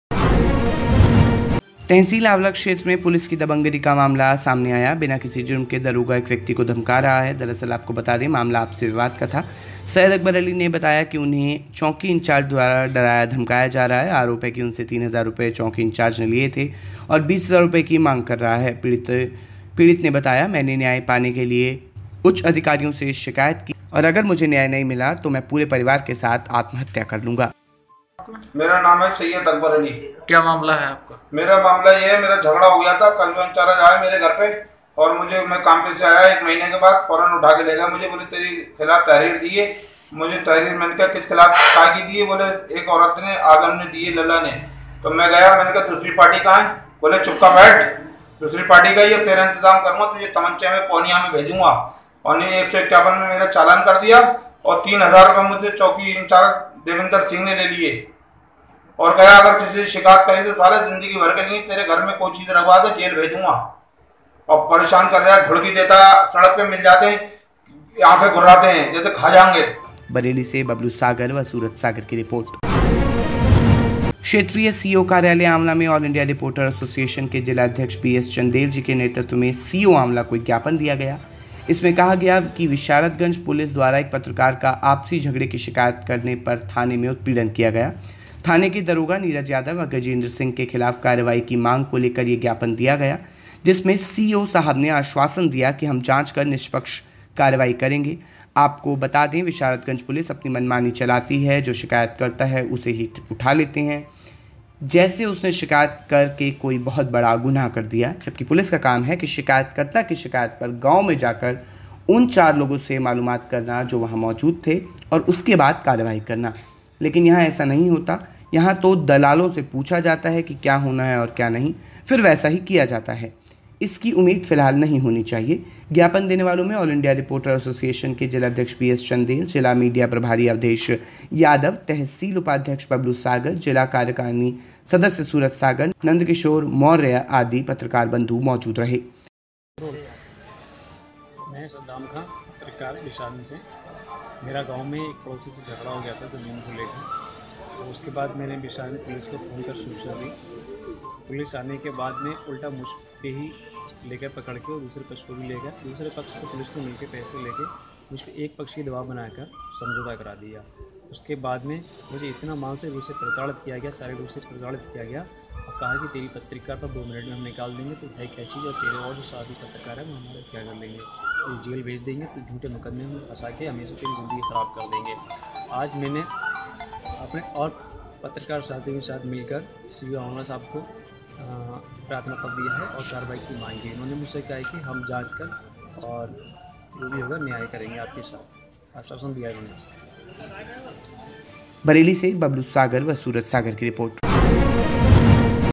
SPEED NEWS : चौकी इंचार्ज पर बीस हजार रूपये मांगने का आरोप
तहसील आंवला क्षेत्र में पुलिस की दबंगिरी का मामला प्रकाश मे आया है | बिना किसी जुर्म के दरोगा एक व्यक्ति को धमका रहा है दरअसल आपको बता दें मामला आपसी विवाद का था, सय्यद अकबर अली ने बताया कि उन्हें चौकी इंचार्ज द्वारा डराया धमकाया जा रहा है आरोप है कि उनसे तीन हजार रूपये चौकी इंचार्ज ने लिय थे और बीस हजार रूपये की मांग कर रहा है |